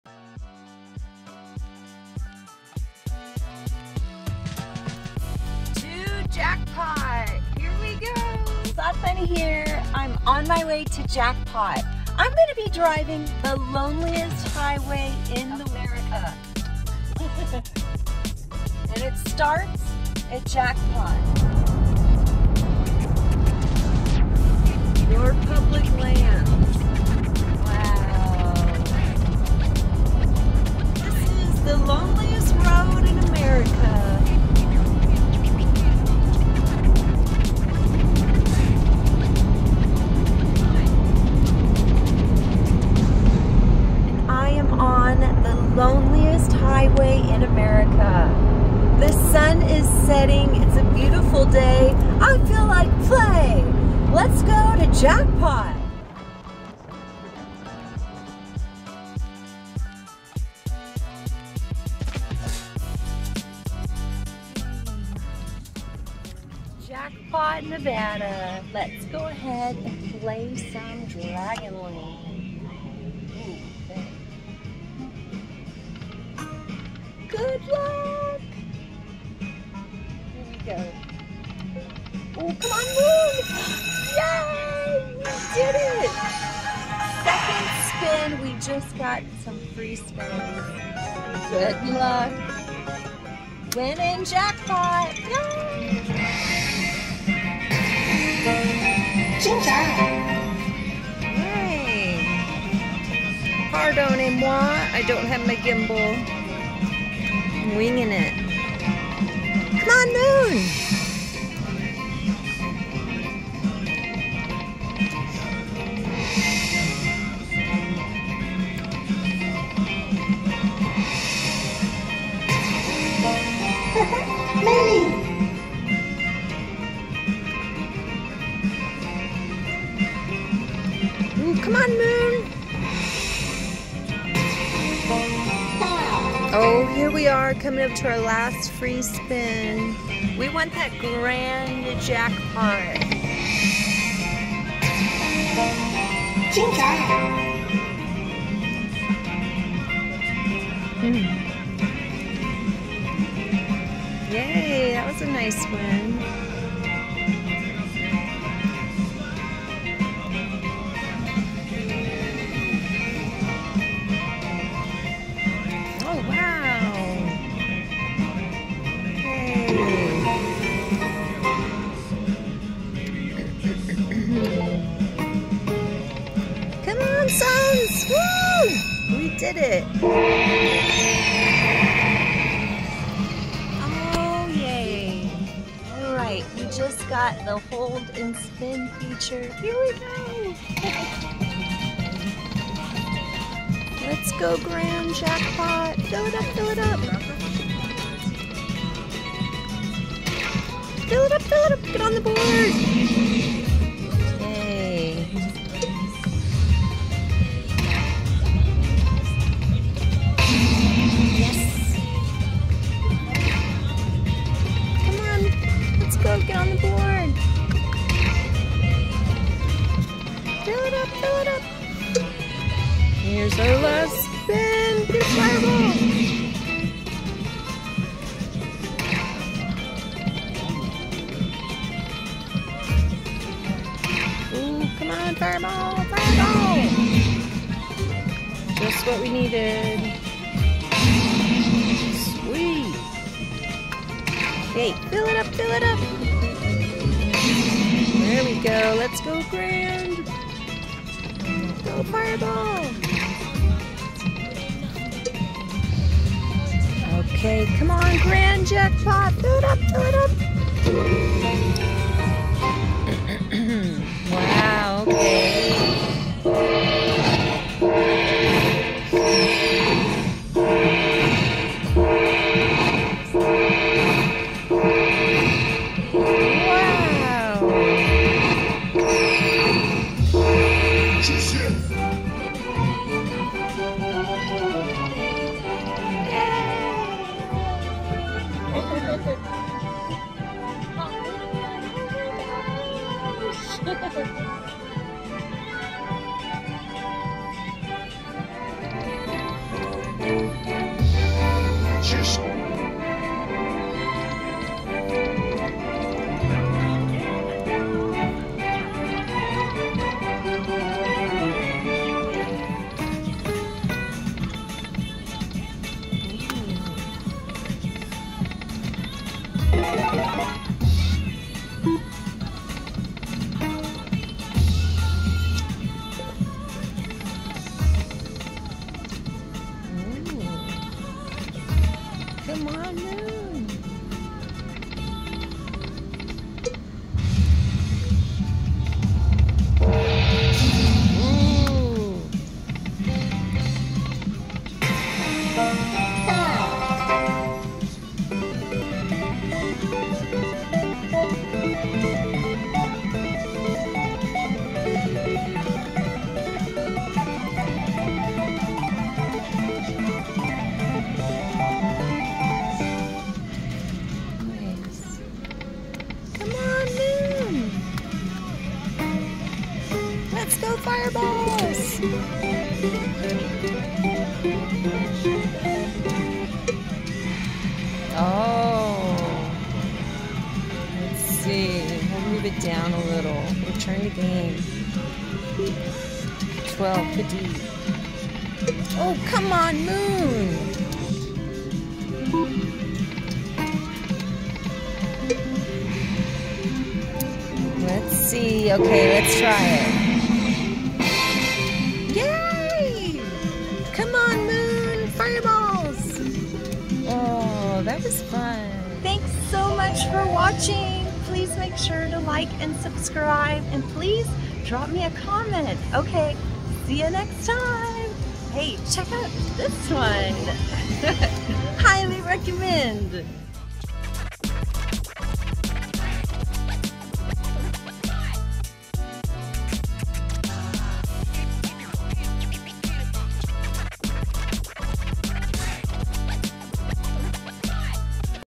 🎰 Slot Bunny's High-Stakes Road Trip to Jackpot, NV! $25 Spins on Dragon Link at Cactus Pete’s!
Hey, it's Slot Bunny! Join me on an unforgettable journey along the Loneliest Highway in America, Highway 93, heading to Jackpot, Nevada. Marvel at the stunning scenery as I drive north and finally arrive at Cactus Pete’s. There, I dive into high-limit action on the Dragon Link slot game with $25 spins. 🙌😎

Inspired by Cowboy Slots Brantley and Marc's recent trip and after running into Brantley at Nugget Wendover, where he told me to have a blast, I’m ready for the challenge!

Will I hit a jackpot in Jackpot? Tune in to experience the high-stakes thrills and the excitement of the road trip and the game, even if luck wasn’t on my side this time!